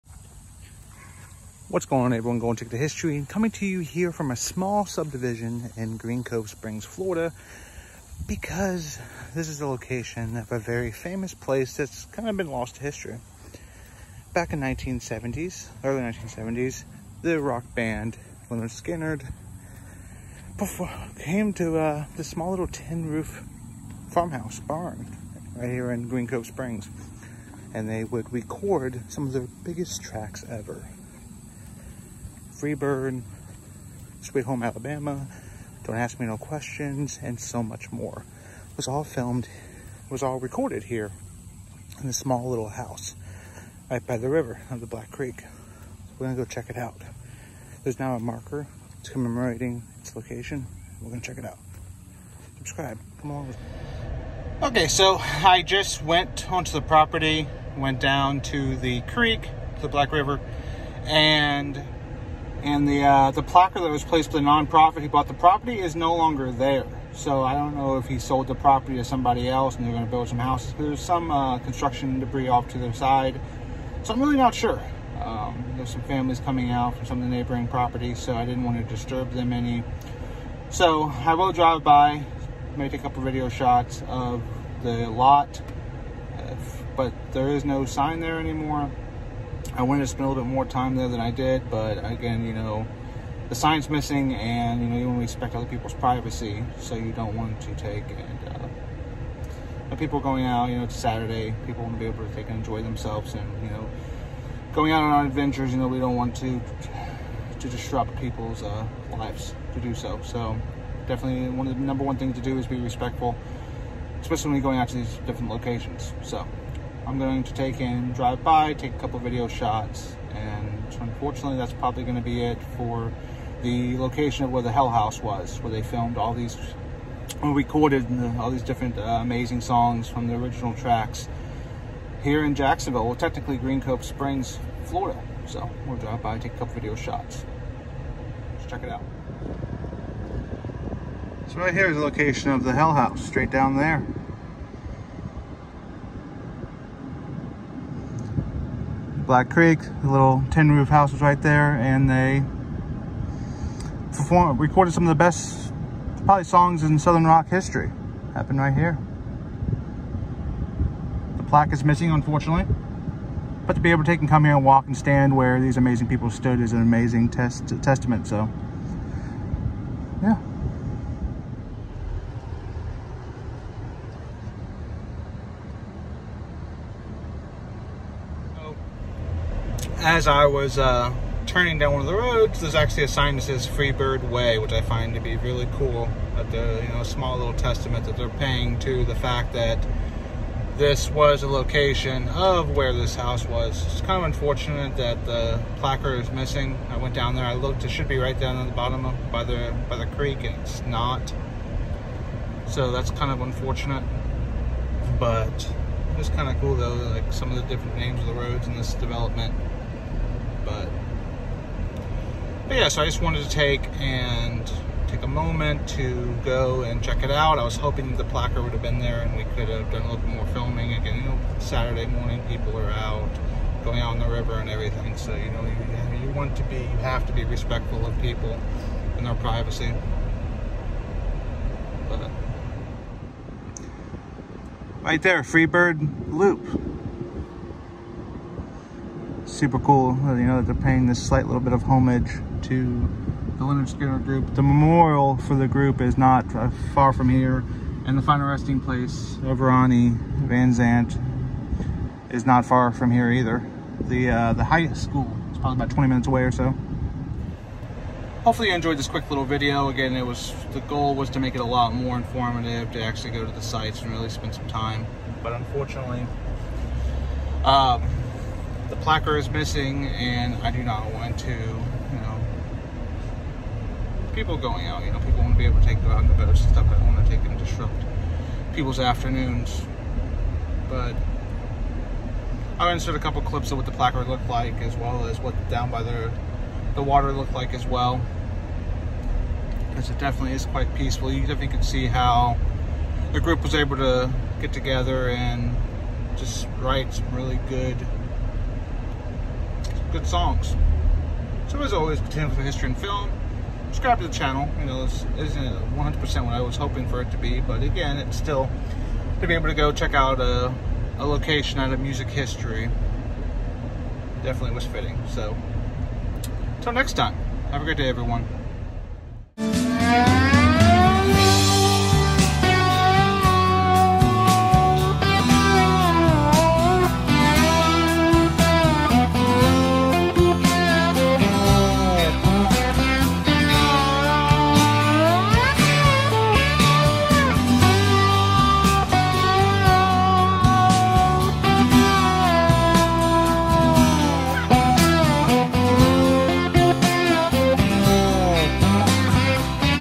Real Life Location of LYNYRD SKYNYRD Hell House Then and Now
I visit the real life location of the hell house just outside of Jacksonville Florida where lynard skynard wrote their first two albums